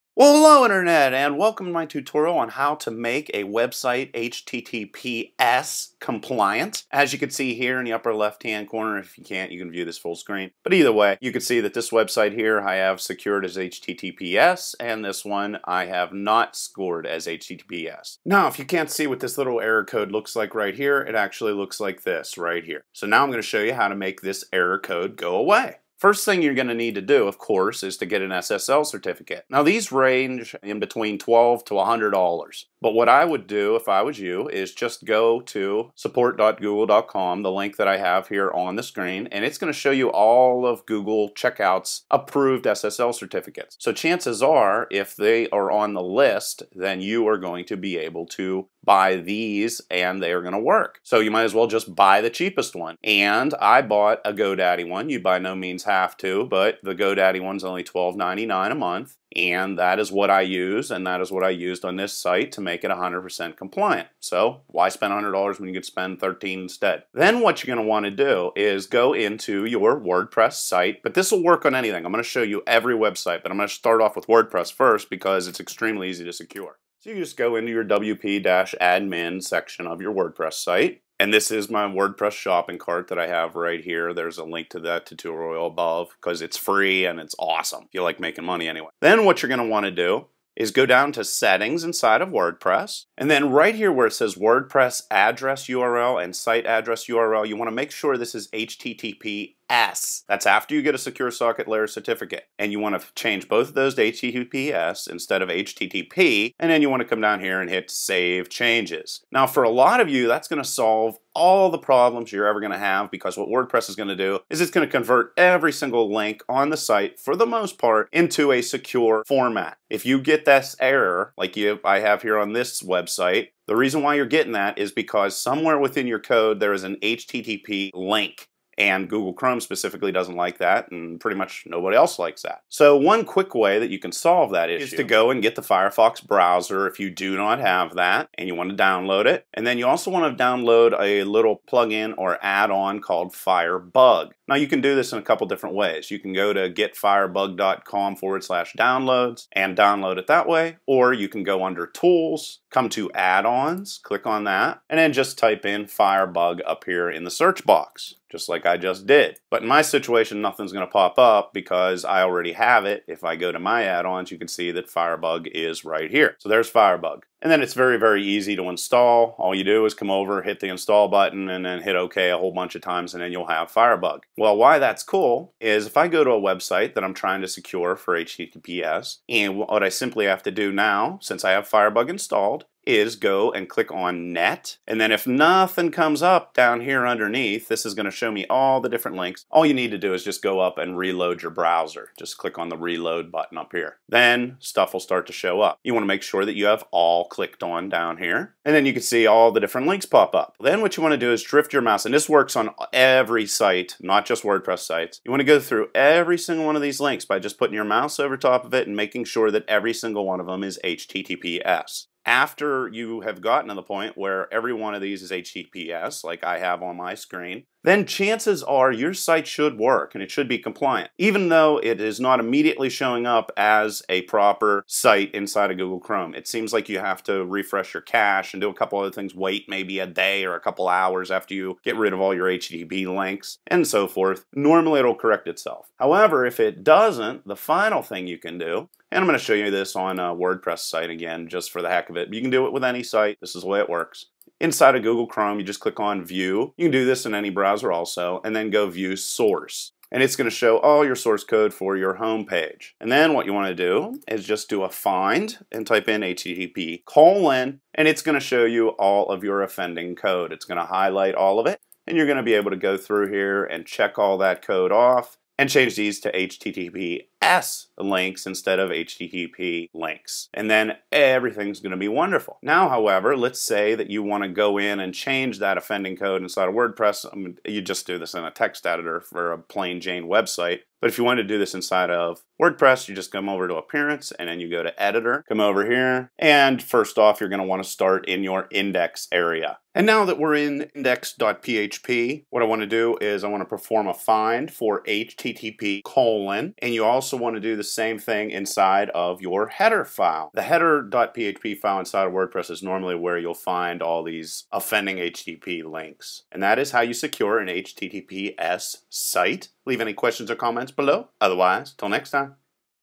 WordPress HTTPS
In this tutorial, I'll show you how to setup a Wordpress HTTPS site, fix Google HTTPS errors, get the cheapest SSL certificate and more.